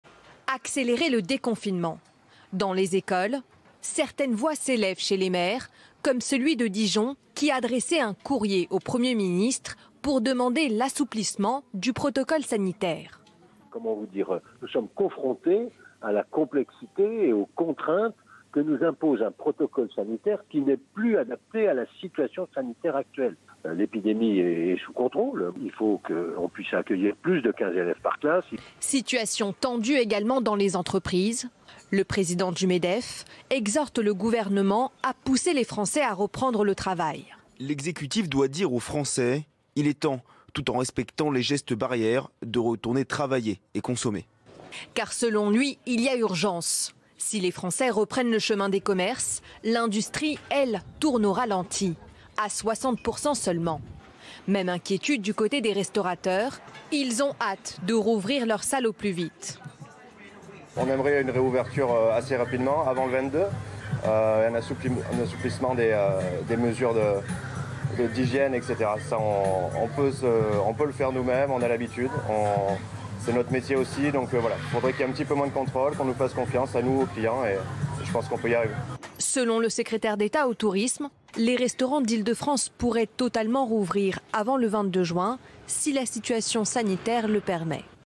Déconfinement : l'exécutif sous pression
Accélérer le déconfinement. Dans les écoles, certaines voix s'élèvent en ce sens chez les maires. C'est le cas de François Rebsamen, maire de Dijon, qui a adressé un courrier au Premier ministre pour demander l'assouplissement du protocole sanitaire.
Situation tendue également dans les entreprises. Geoffroy Roux de Bezieux, le président du Medef, exhorte le gouvernement à pousser les Français à reprendre le travail.